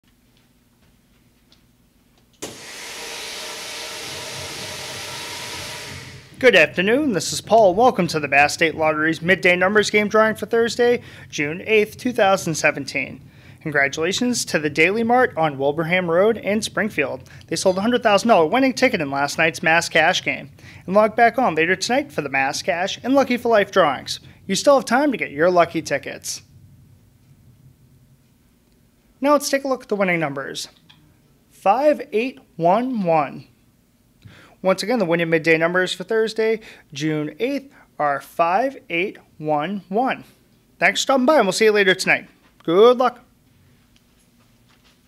Midday Numbers Game Drawing: Thursday, June 8, 2017
The official winning numbers in the Mass. State Lottery's Midday Numbers Game drawing for Thursday, June 8, 2017 are: 5-8-1-1.